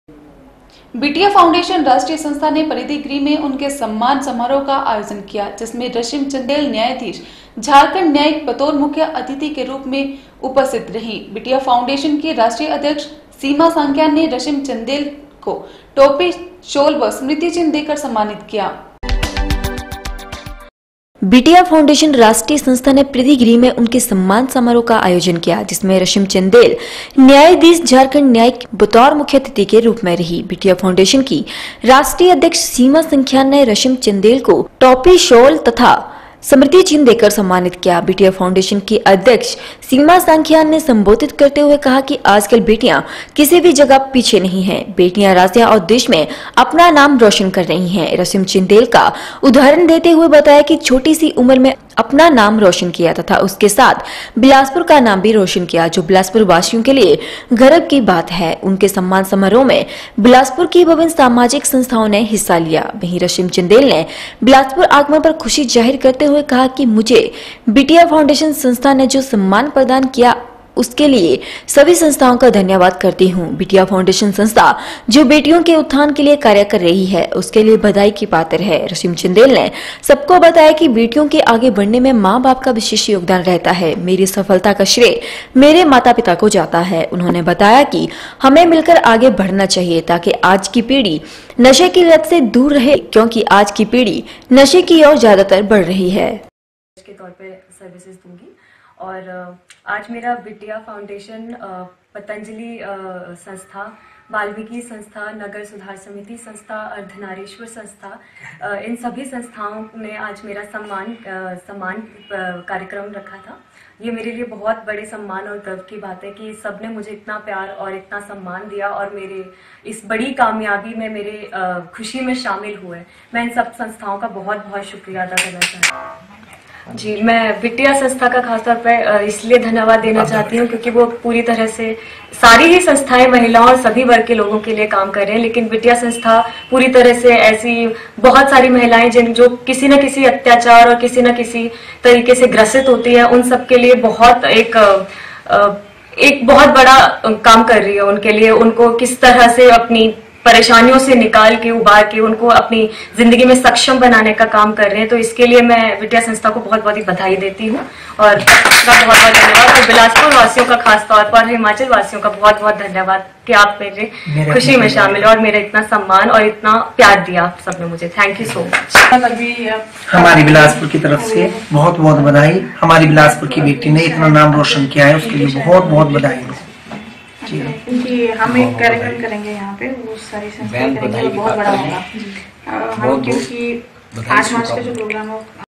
4 MARCH N 10बिटिया फाउंडेशन राष्ट्रीय संस्था ने परिधि गृह में उनके सम्मान समारोह का आयोजन किया
बिटिया फाउंडेशन राष्ट्रीय संस्था ने परिधि गृह में उनके सम्मान समारोह का आयोजन किया गया जिसमे रशिम चंदेल न्यायाधीश झारखंड न्यायिक बतौर मुख्य अतिथि के रूप रही बिटिया फाउंडेशन की राष्ट्रीय अध्यक्ष सीमा संख्यान ने रशिम चंदेल टोपी शॉल तथा स्मृति चिन्ह देकर समानित किया गया